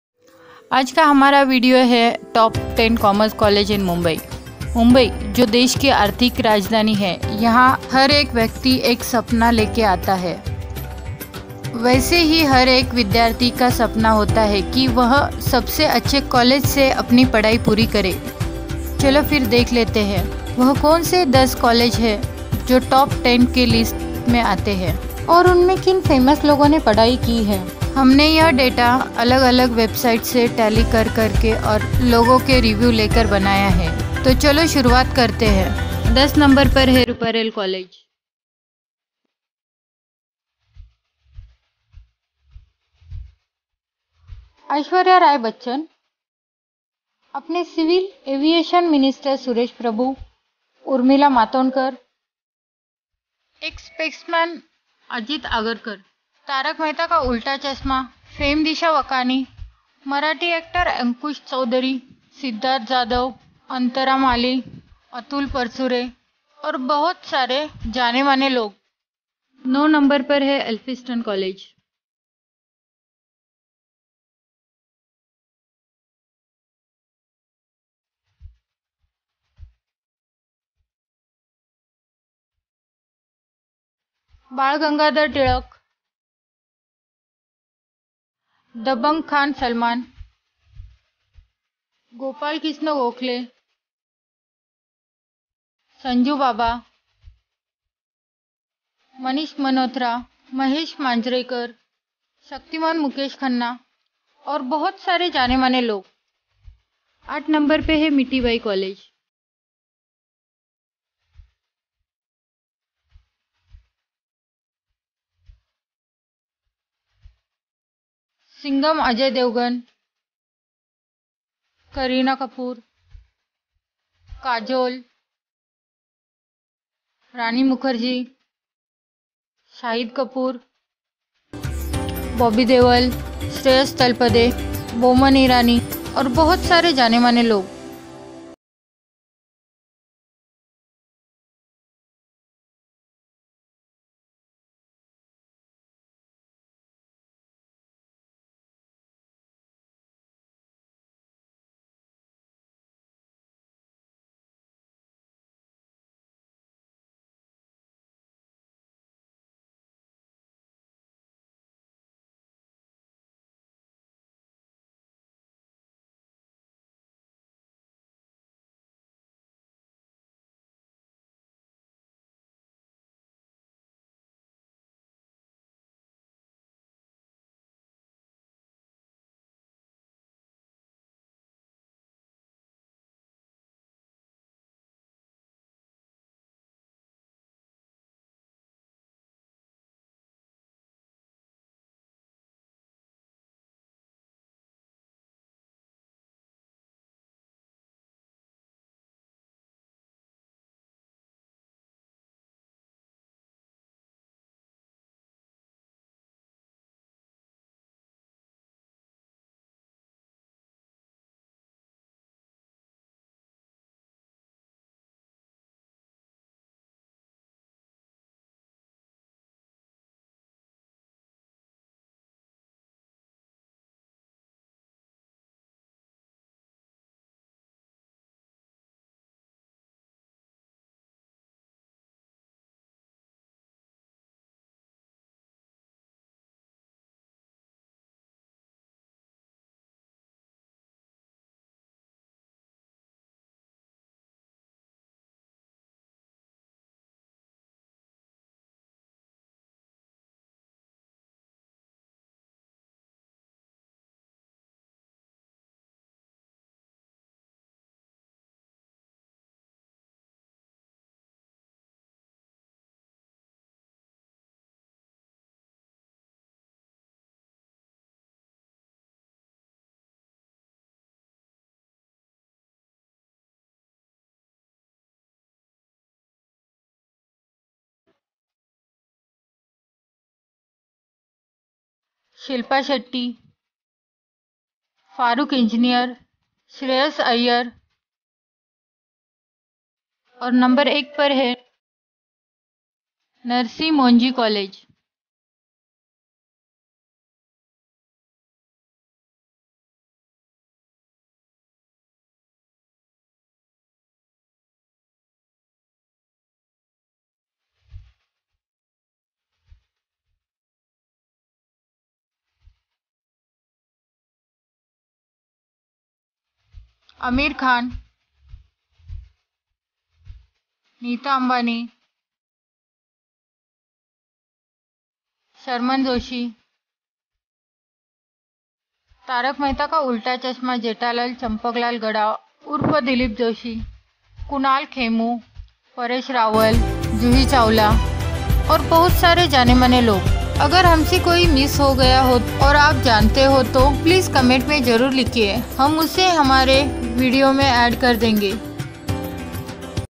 Top 10 Commerce Colleges in Mumbai & Celebrities Studied
This video is about top 10 commerce colleges in mumbai & celebrities studied in those colleges. the rating is purely based on data available online. this video is just for entertainment purpose